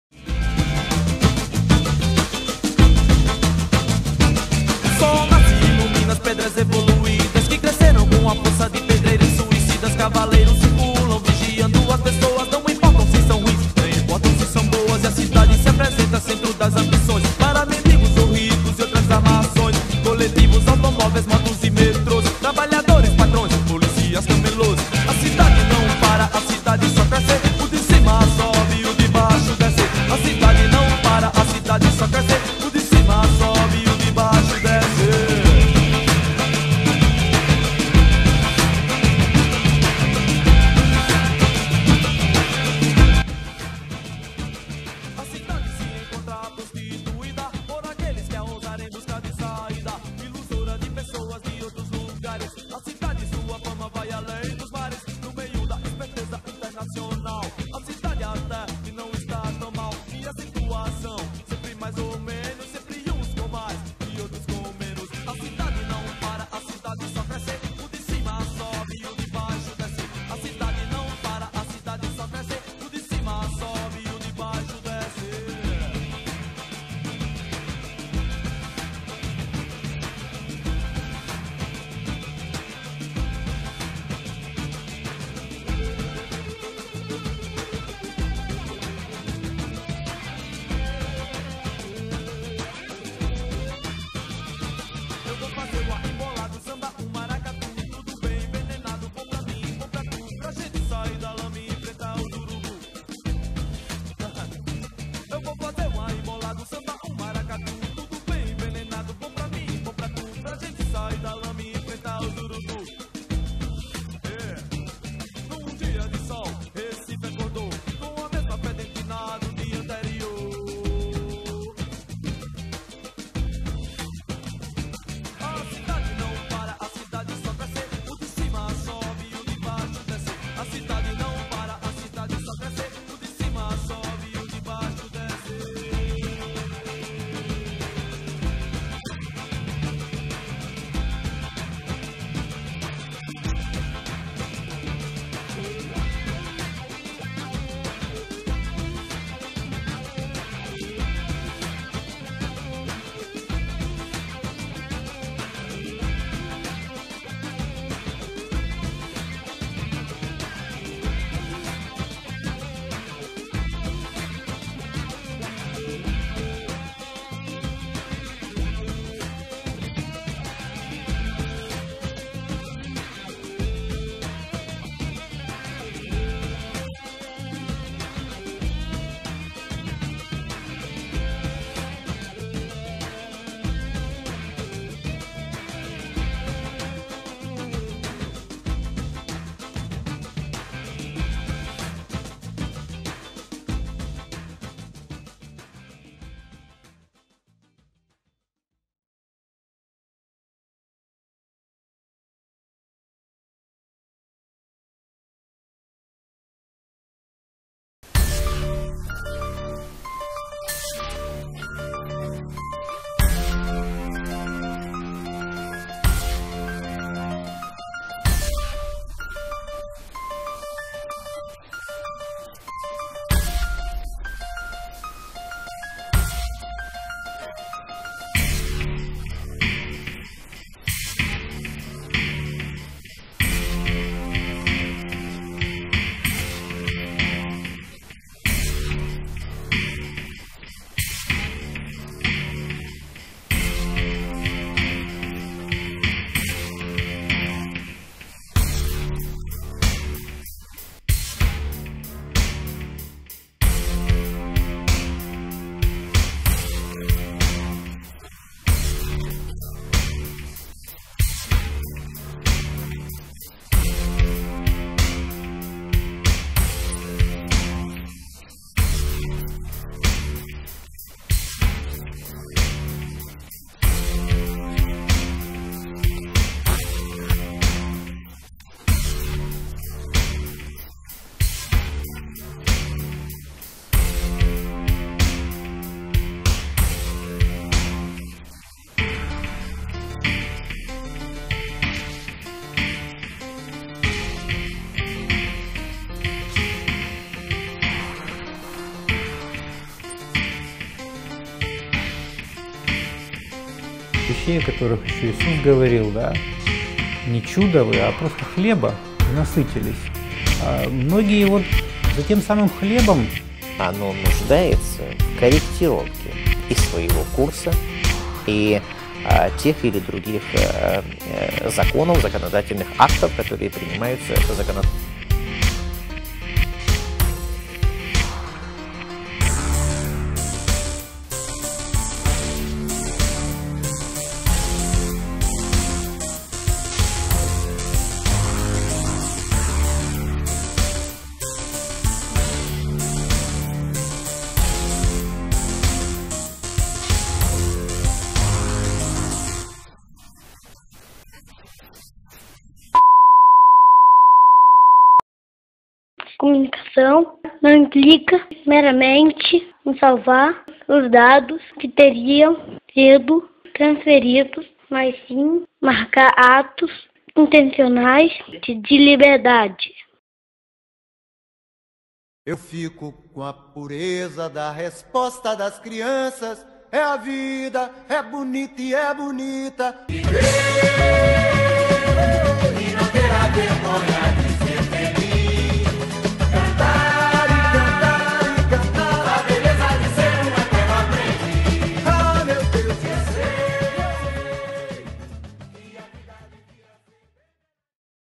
Doc "Liberdade?" (trabalho acadêmico)
Mini-documentário mesclando opiniões reais com partes fictícias sobre o tema "liberdade". Executado por alunos do curso de Comunicação em Mídias Digitais (UFPB), em 2013.